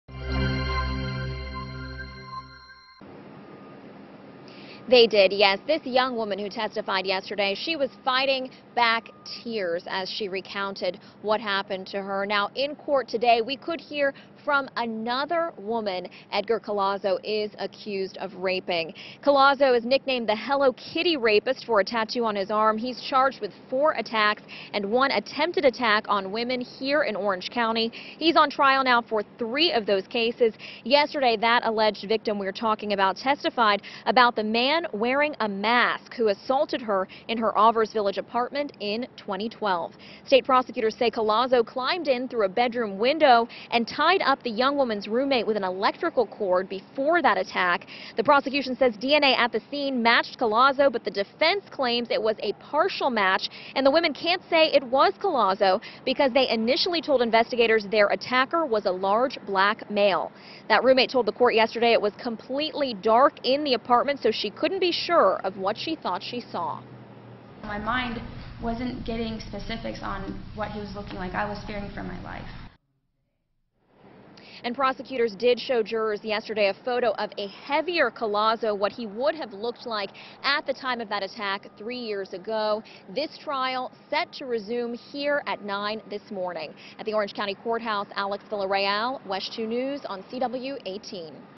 Second alleged victim could testify in rape trial
The jury heard from an alleged victim Tuesday in the trial of Edgar Collazo, also known as the alleged "Hello Kitty" rapist.